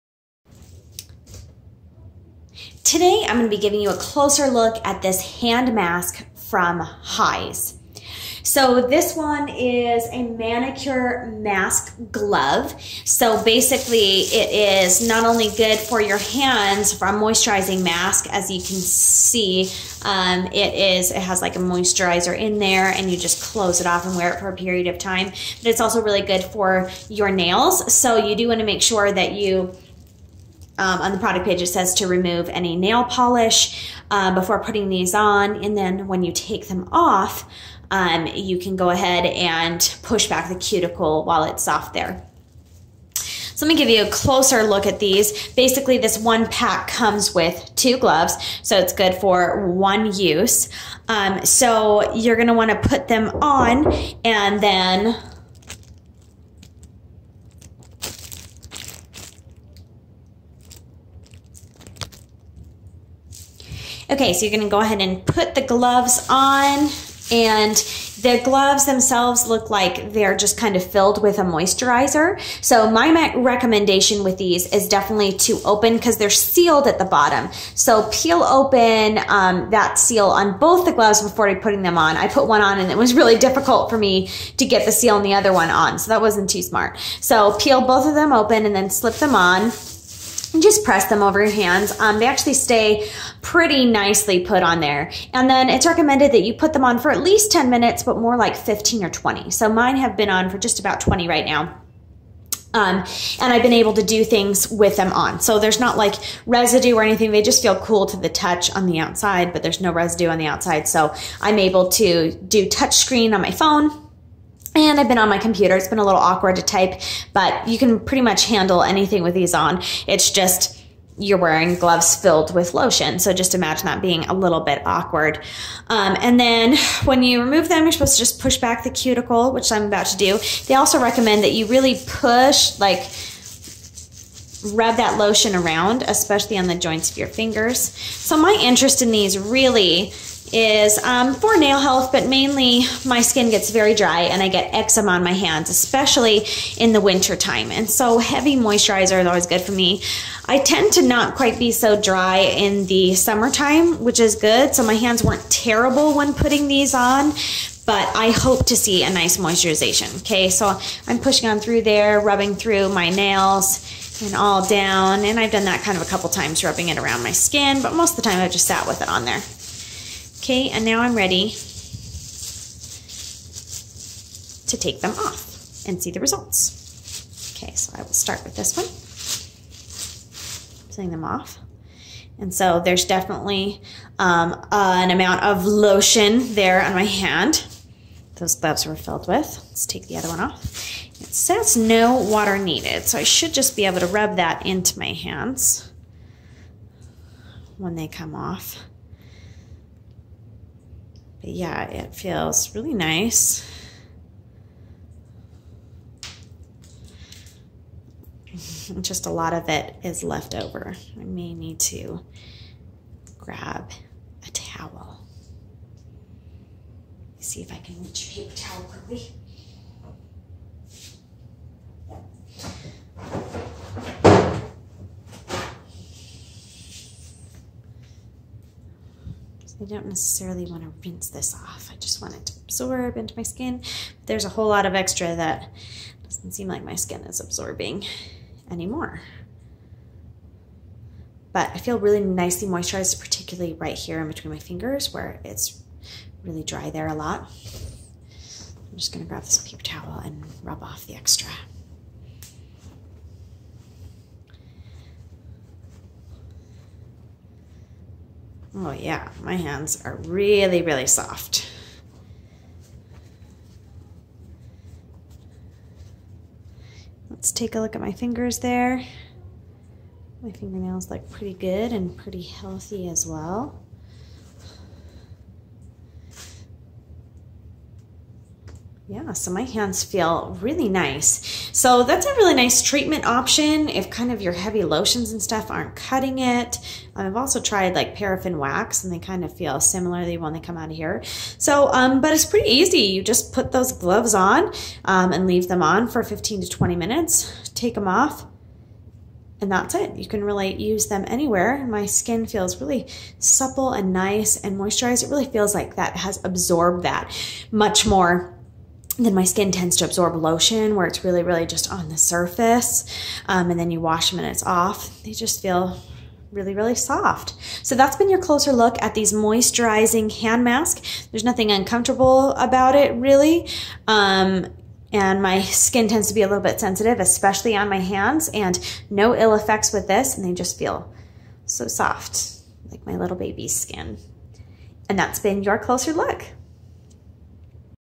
HIGH'S Nutritive Home Spa Manicure Hand Mask Moisturizing Gloves
To View or Purchase Product

This is a pretty cool manicure mask gloves. It is good for both your hands and nails for making them soft, healthy and smooth. These are basically plastic gloves filled with lotion that you pull on for anywhere from 10-20 minutes. I used mine for a full 20 and it was pretty nice that I was able to type on my computer and use my touch-screen phone and everything without too much difficulty with them on.

For me, the experience was similar to a paraffin wax dip as far as results go I just didn’t had the hassle of peeling the wax off or not being able to use my hands. It was easy and my hand got a really deep moisturizer.